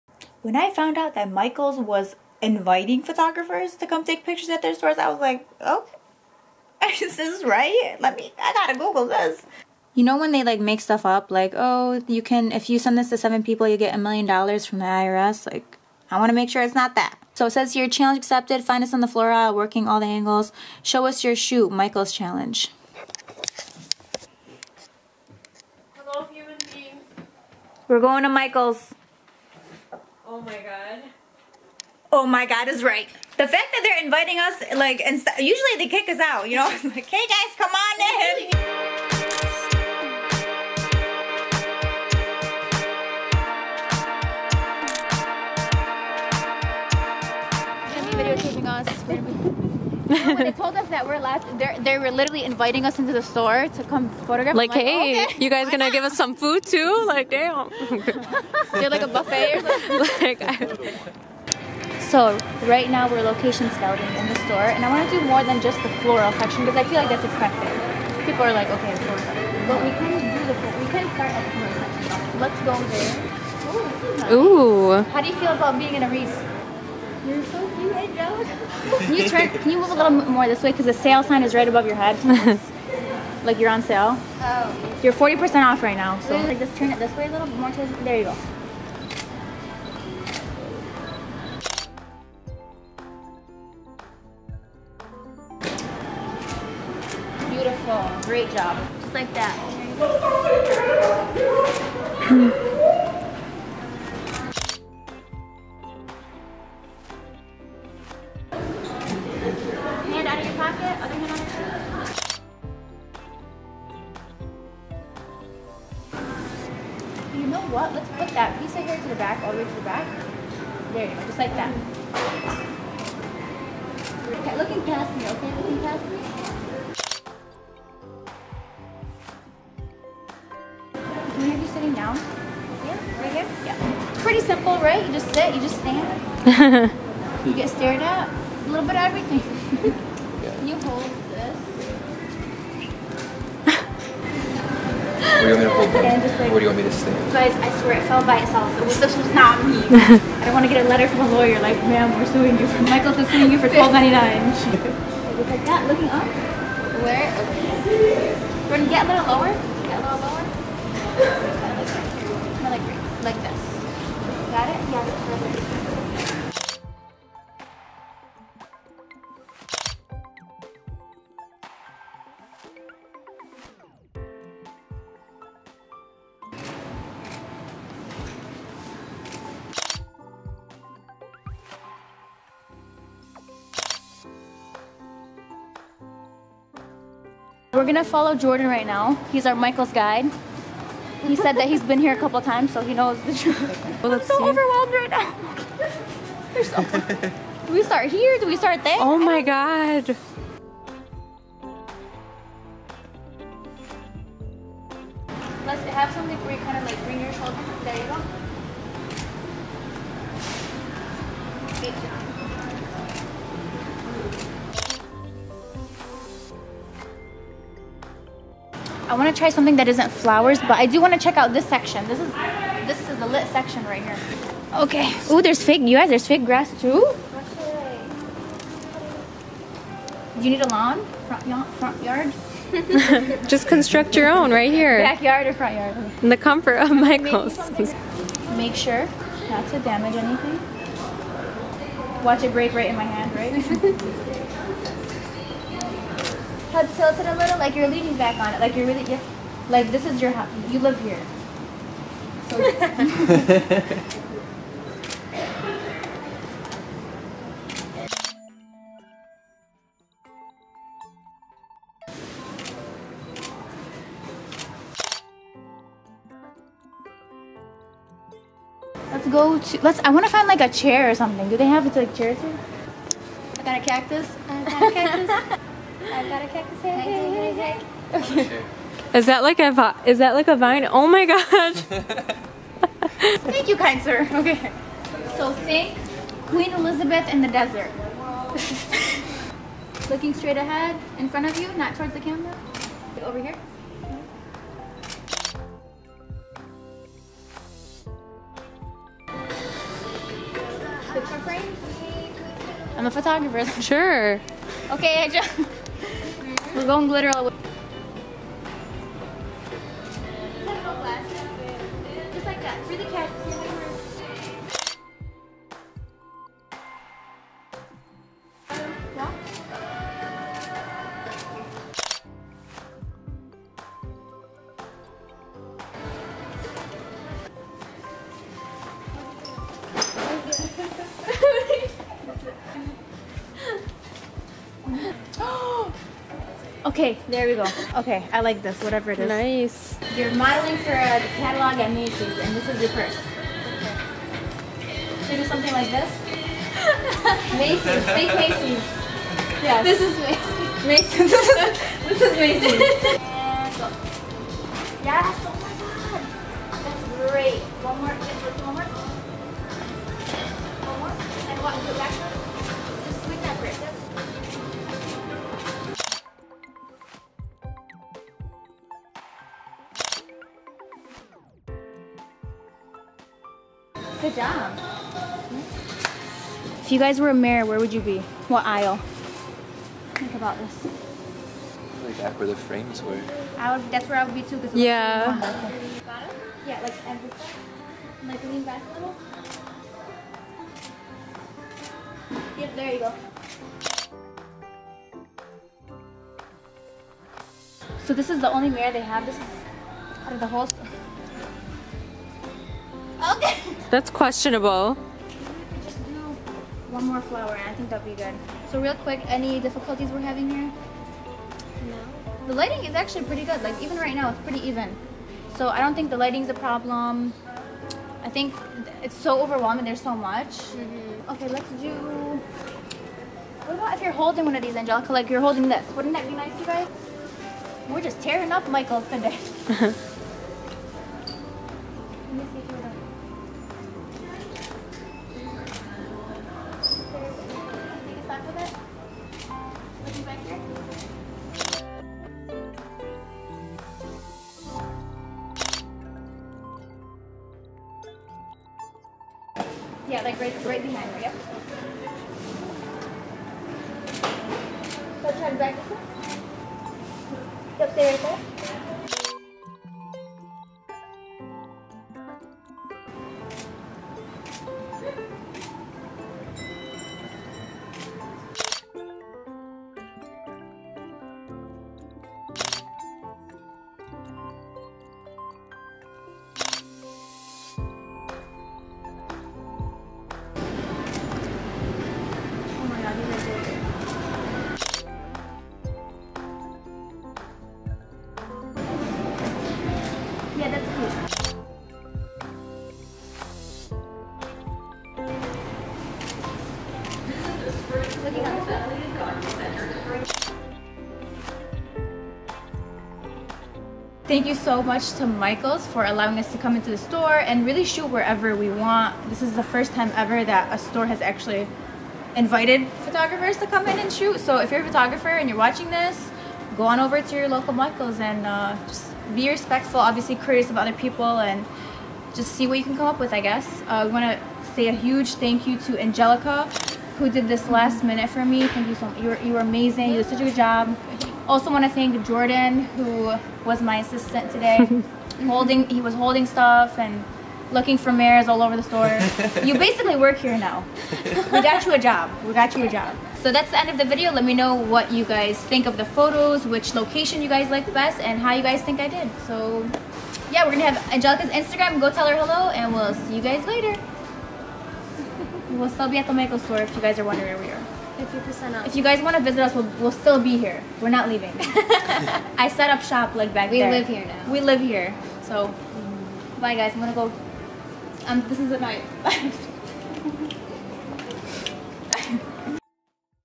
MICHAELS CRAFT STORE PHOTO CHALLENGE
In this video I take photos at a local Michaels craft store.